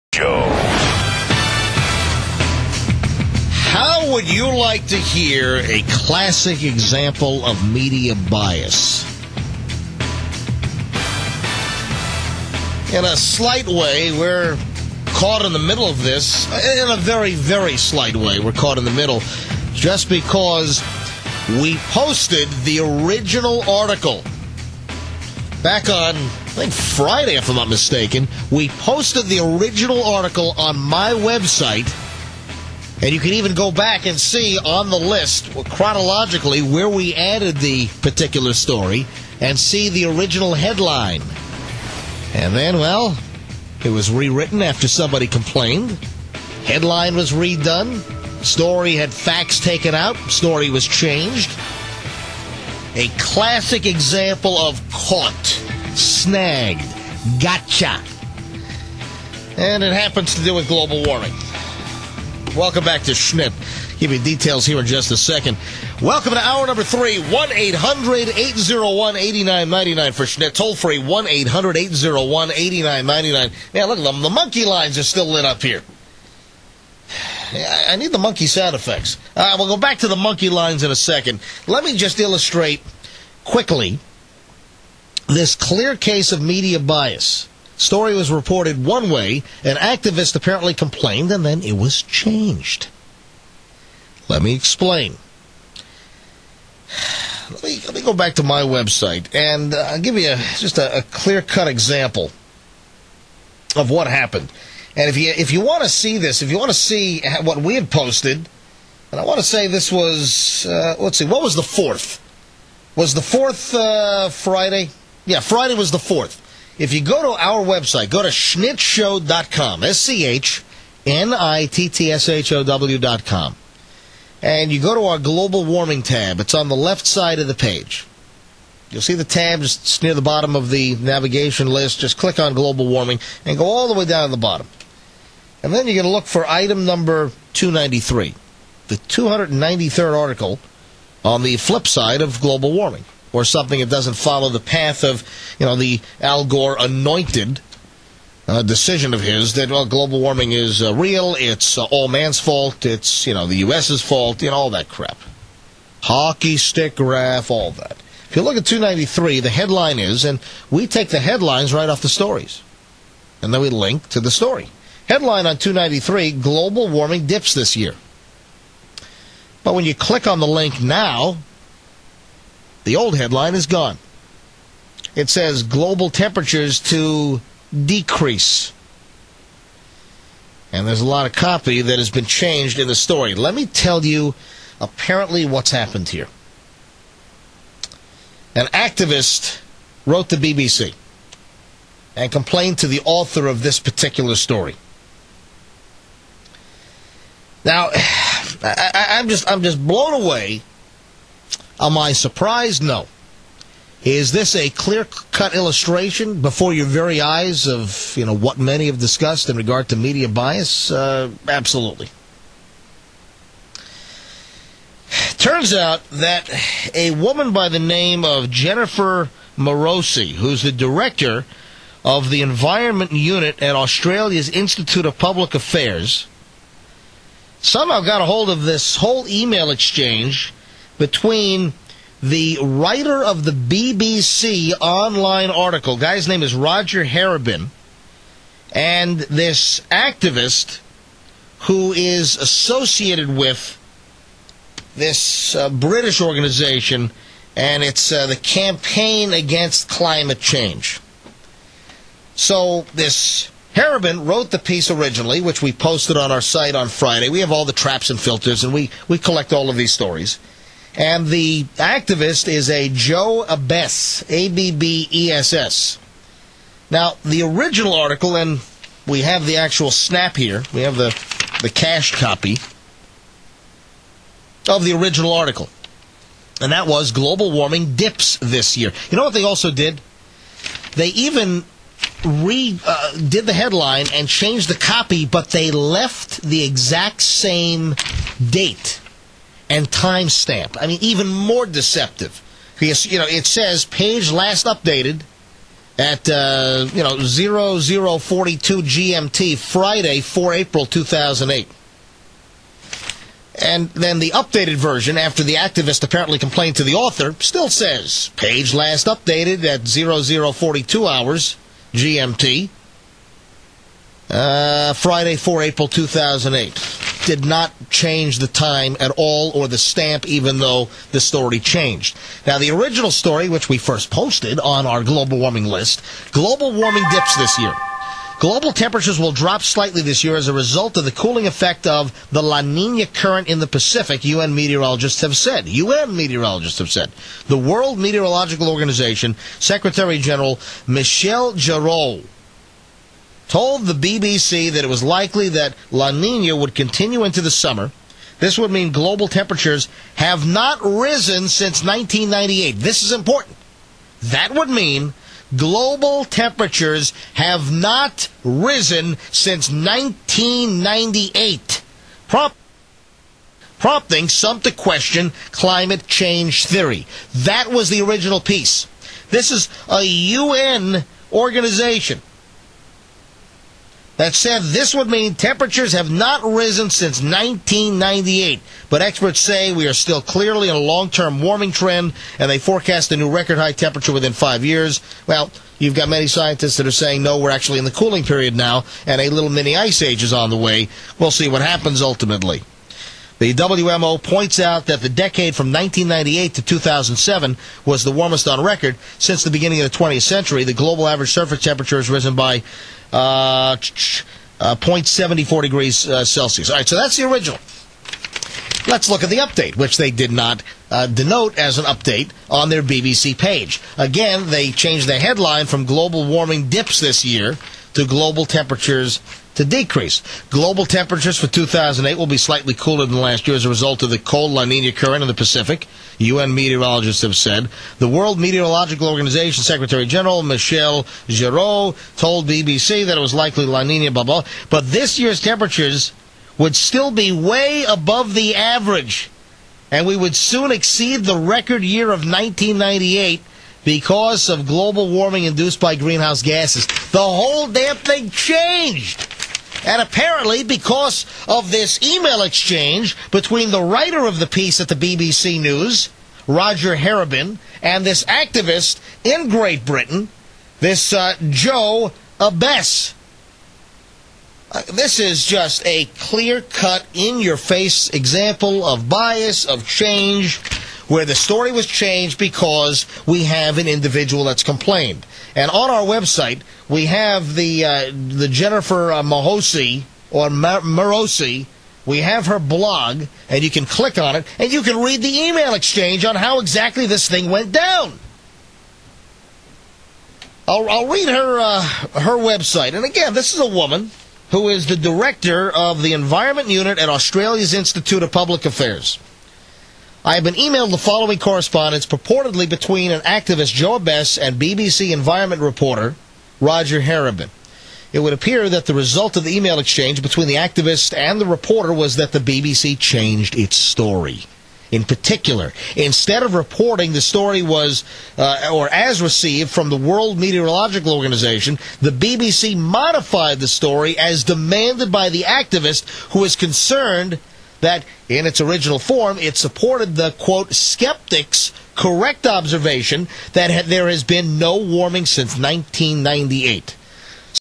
BBC changed article because of Activists DEMANDS
BBC wrote of the Global cooling changes of the past few years and a Global Warming activist wrote and demanded that they change the article.
So she did.
And not only did she change it, but she also left the "edited" date as the same as the original article.
listed the original article (along with date stamp) and then posted the updated, altered version as well.
This is clearly Media Bias furthering the scam.
Listen and be amazed.
BBC BUSTED !